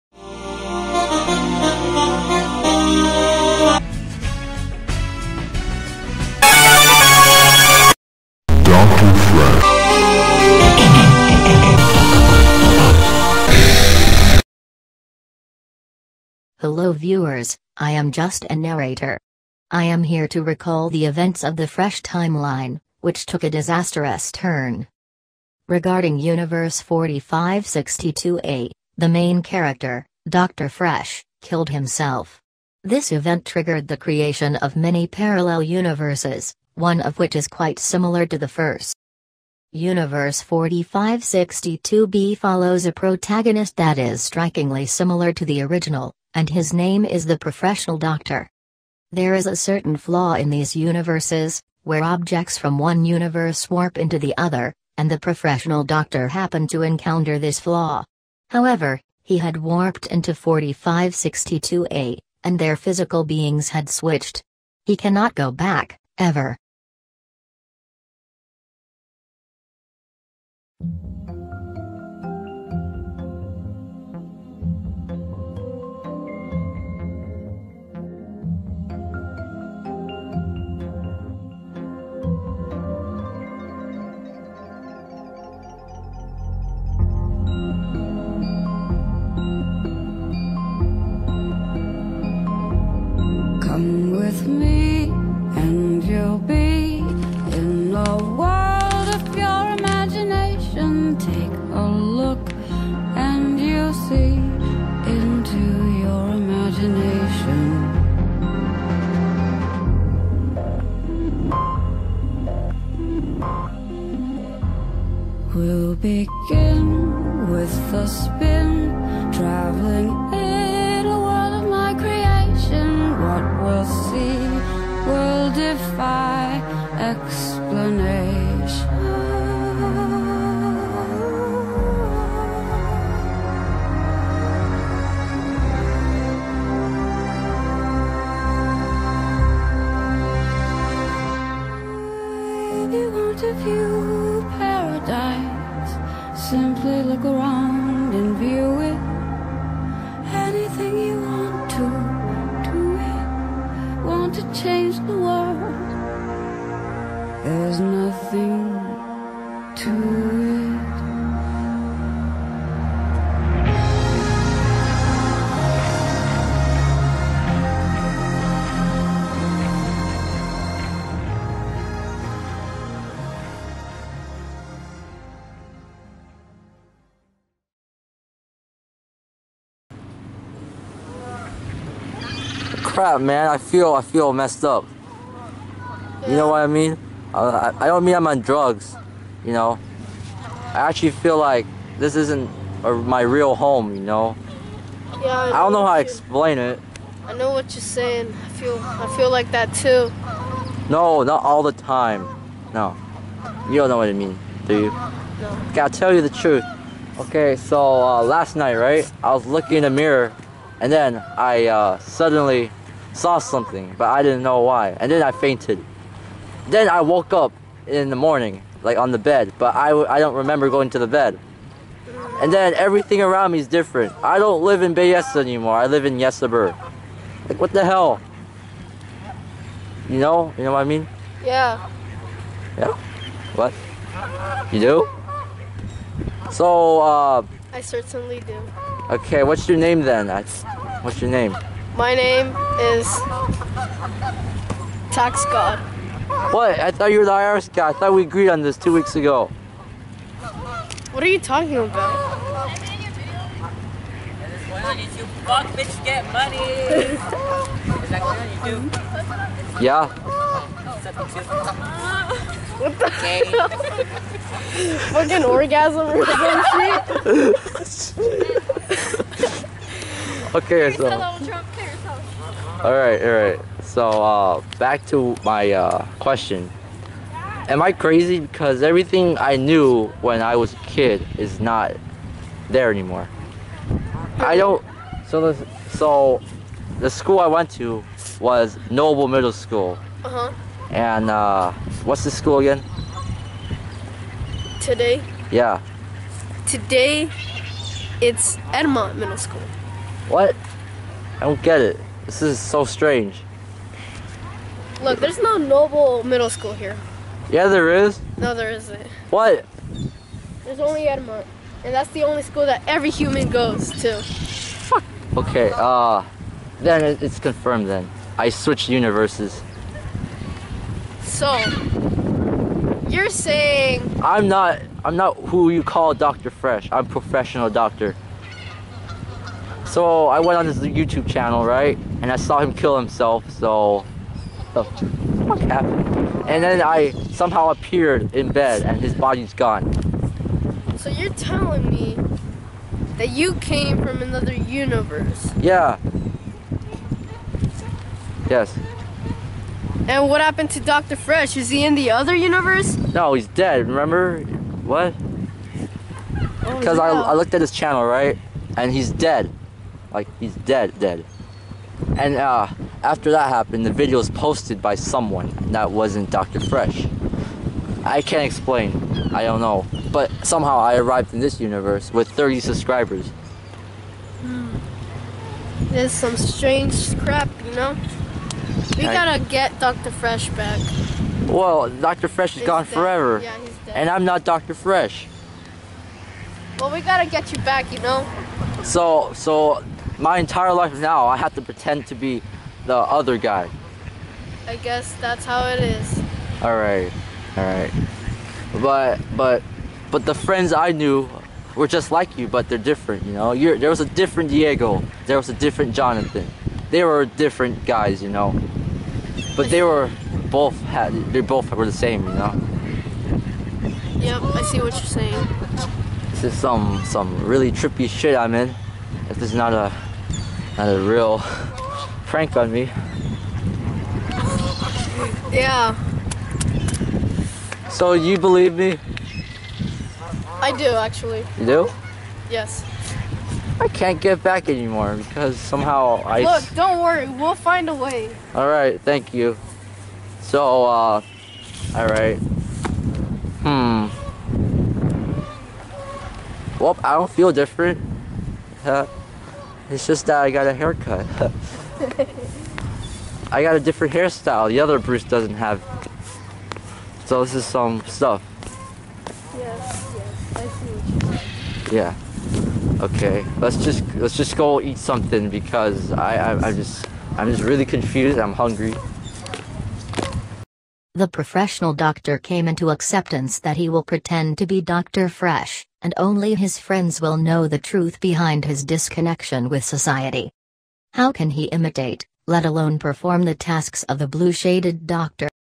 The Switch. (Plot Episode) d0ctor fresh: The Underscorer
After the old d0ctor_fresh's departure, this new "d0ctor" will never live up to his expectations, but no one knows the difference.